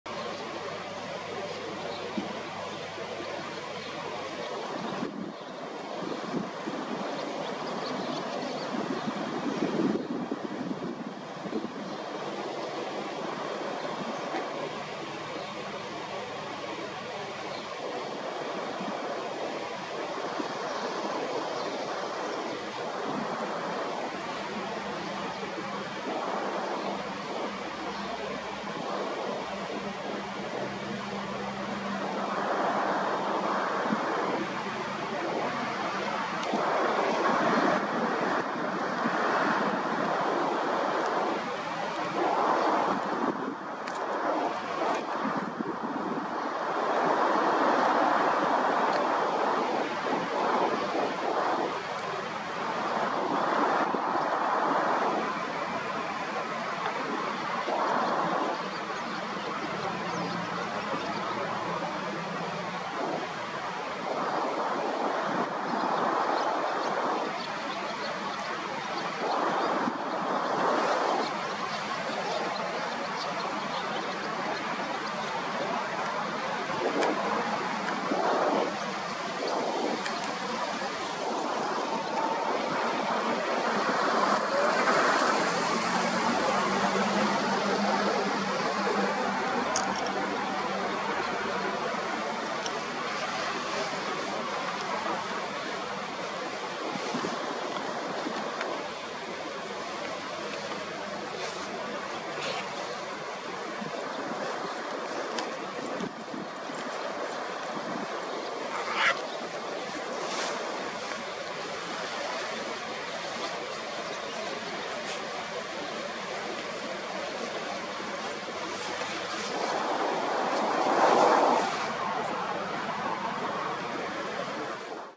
2024 04 11 Barn Swallow - Tweeting and Preening in the morning
撮影日：2024年4月11日（木）
場所：サザンビーチちがさき（神奈川県茅ヶ崎市）
内容：ツバメ　朝の囀りと毛繕い
カメラ：Nikon COOLPIX P1000
再生時間：126秒